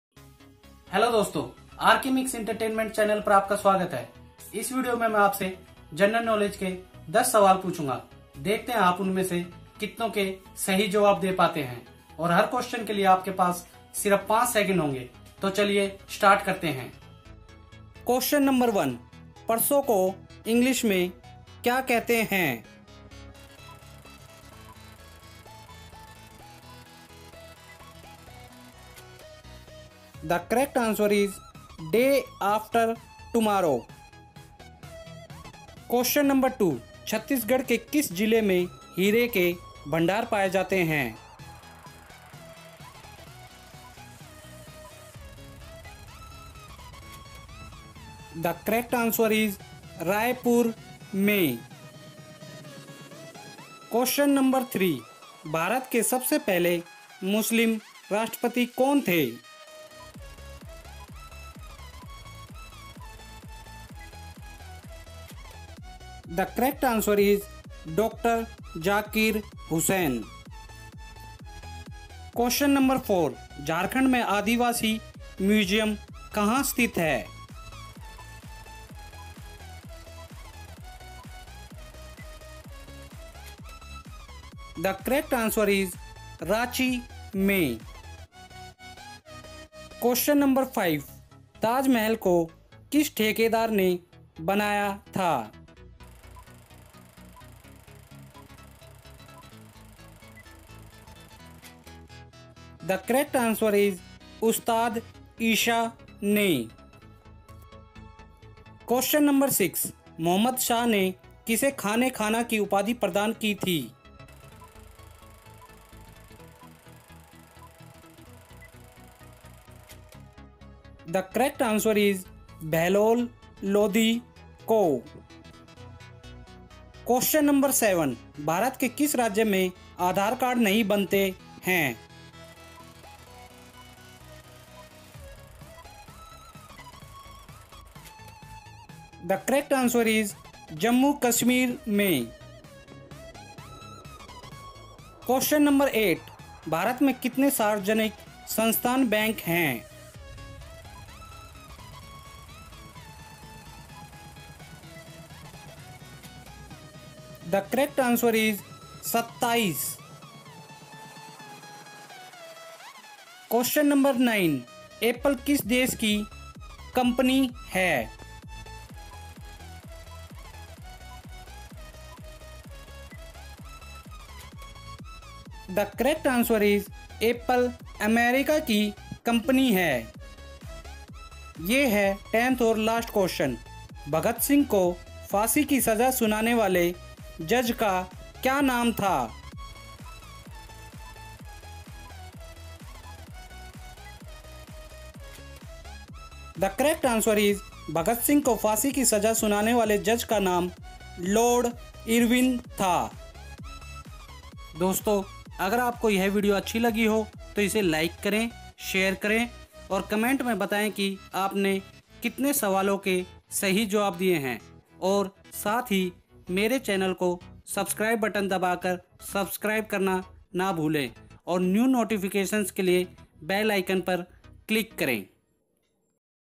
10 Most Important Questions Of General Knowledge || सामान्य ज्ञान के 10 बेहद जरूरी सवाल
This video asks 10 questions of general knowledge which is mandatory for every child who preparing for competitive exams.
Let's see how many you confirmed.